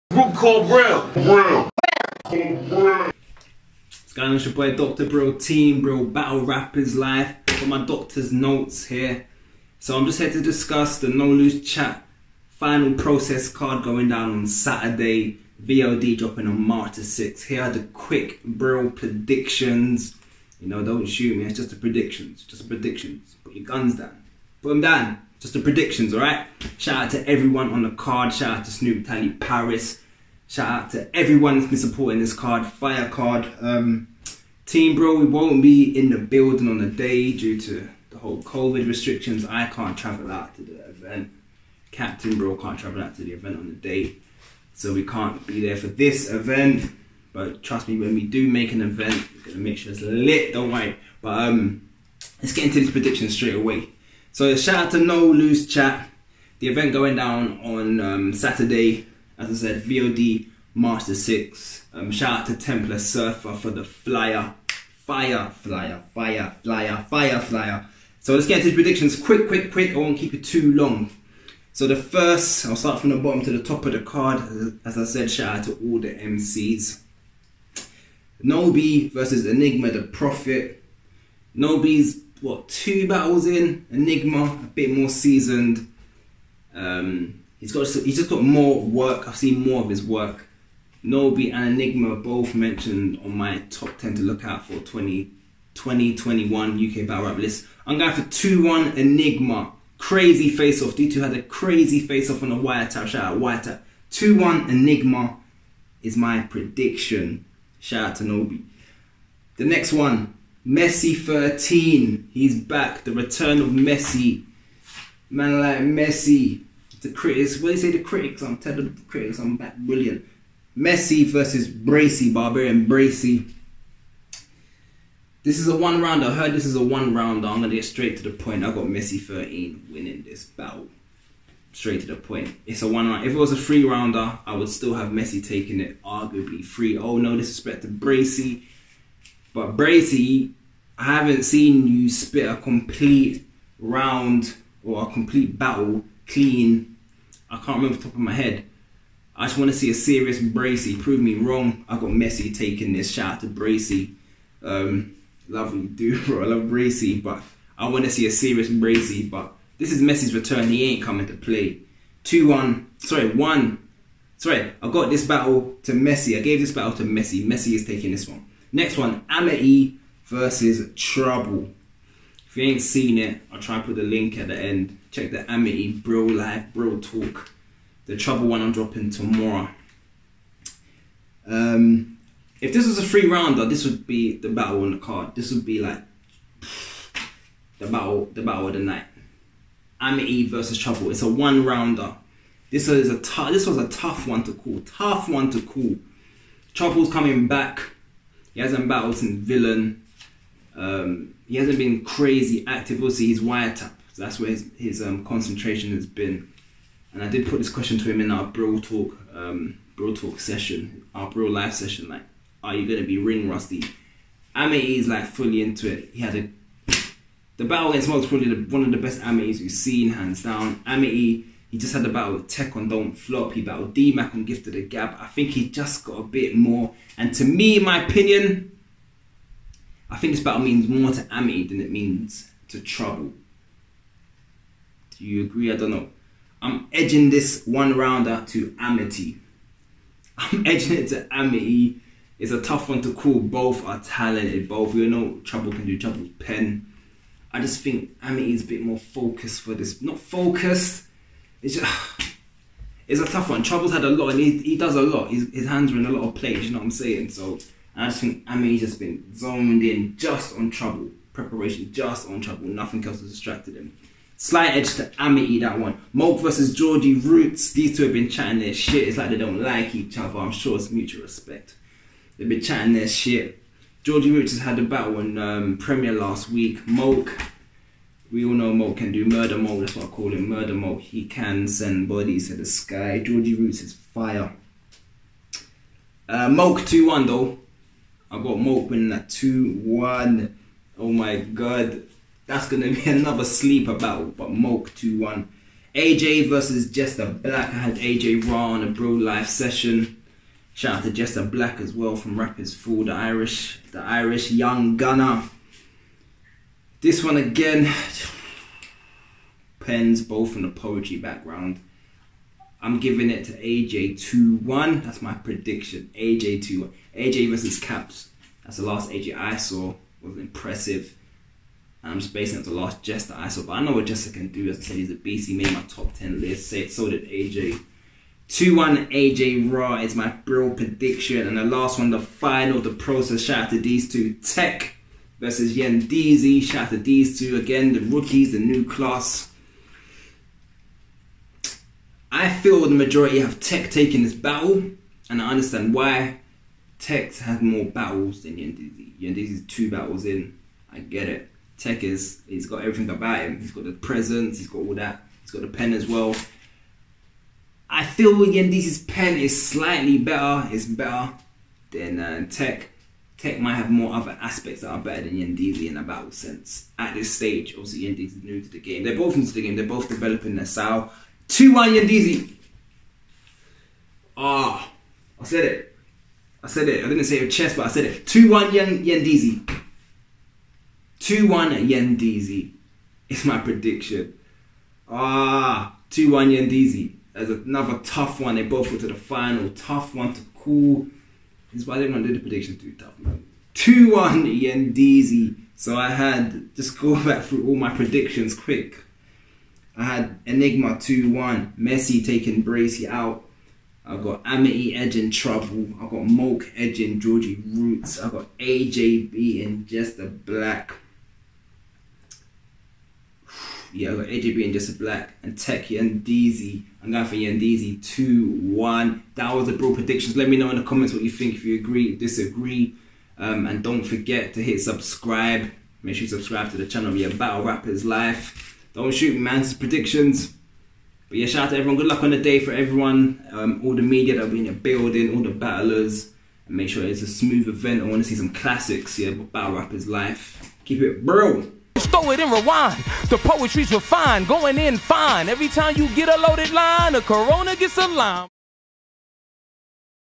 NO LOOSE CHAT FINAL PROCESS BRIL PREDICTIONS
THE BRIL PREDICTION FOR THE UPCOMING NLC FINAL PROCESS CARD

WHO DOES DR BRIL HAVE WINNING EACH BATTLE

VOD OUT MARCH 6TH GO SUPPORT